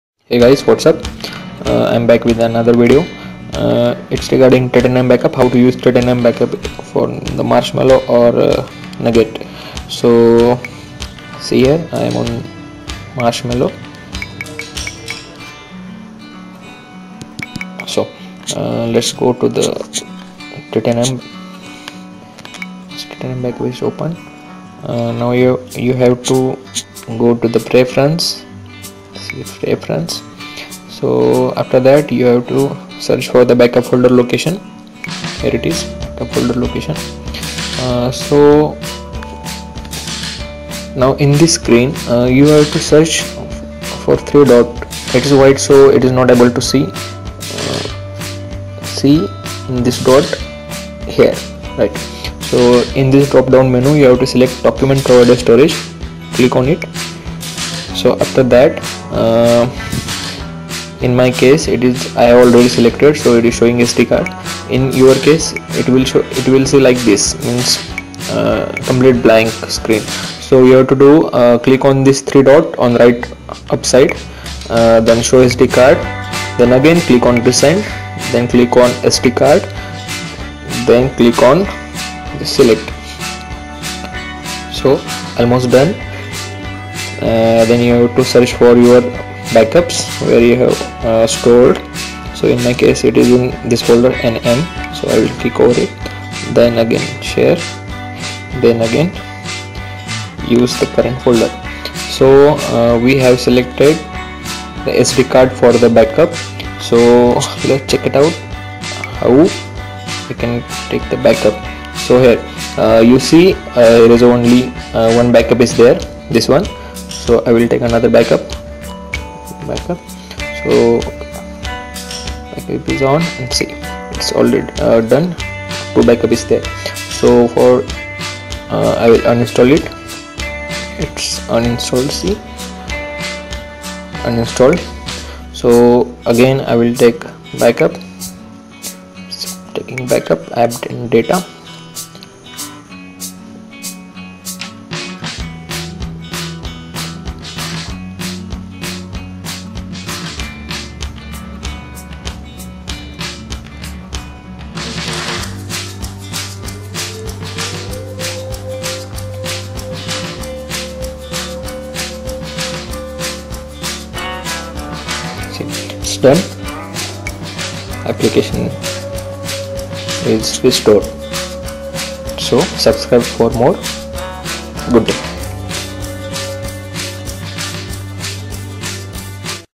NOUGAT TITANIUM BACKUP FIXED
this is a video which tells how to use titanium backup on marshmallow and nougat